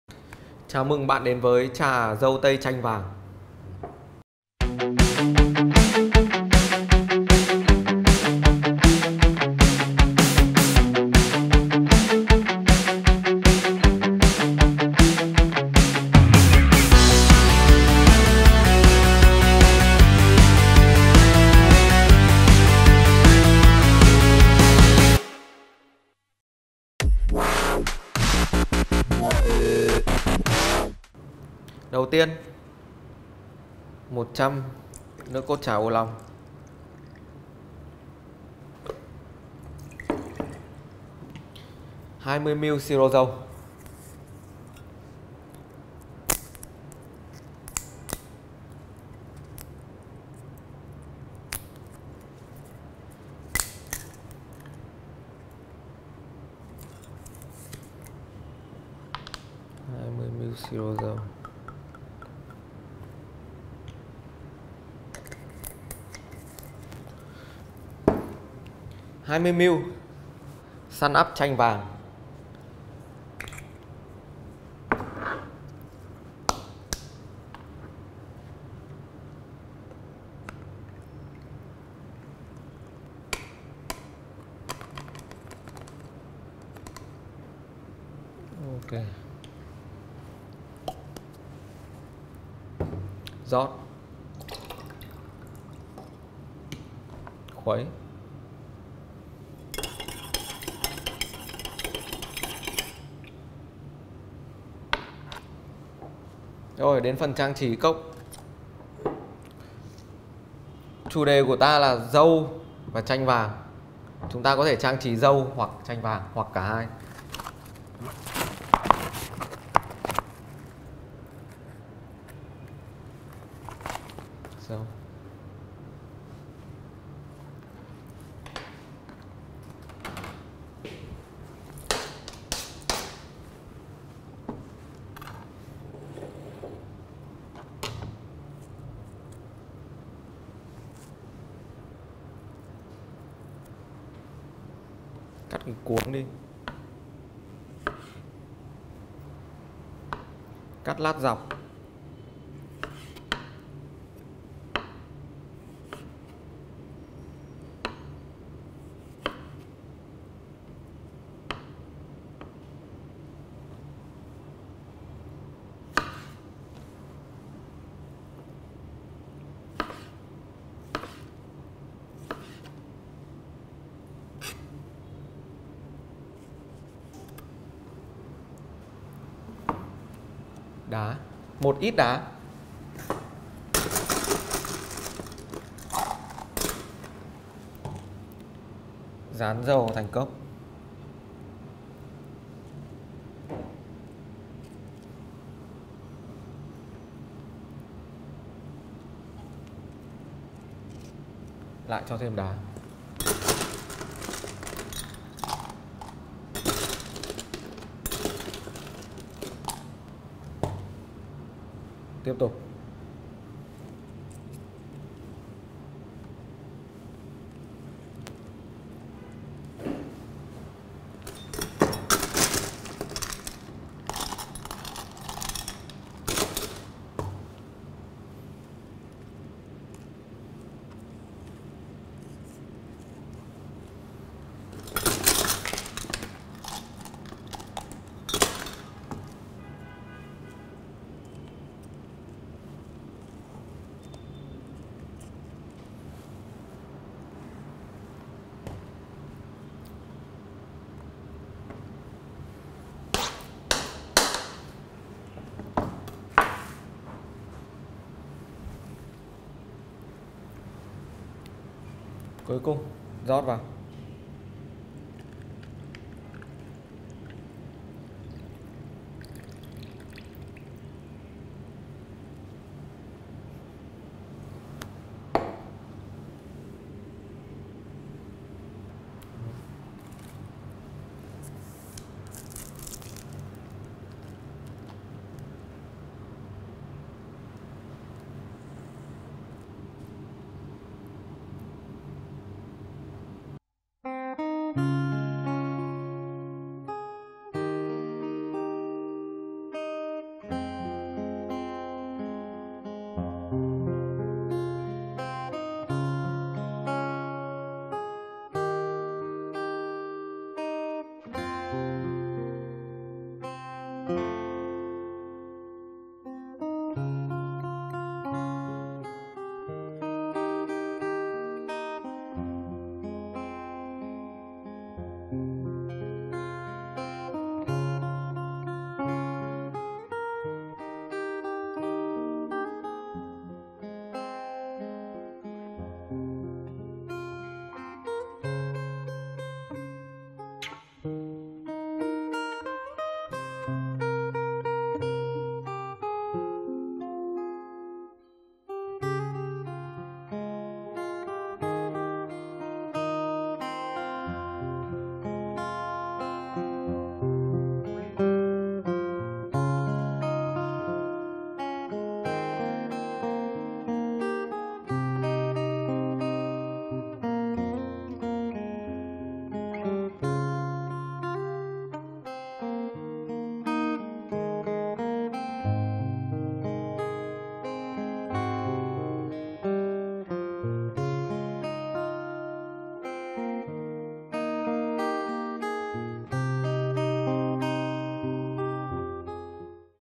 Trà Dâu Tây Chanh Vàng
- Dưới đây là công thức đồ uống có trong video trên:

Trà Xanh Chanh Leo Kimquat Mật Ong

- CÔNG THỨC -
Trà Ô Long  : 100ml
Syrup dâu : 20ml
Mứt Sunup chanh vàng : 20ml
Dâu tây tươi : 2 quả

Cách làm : Cho tất cả nguyên liệu vào bình và lắc lên.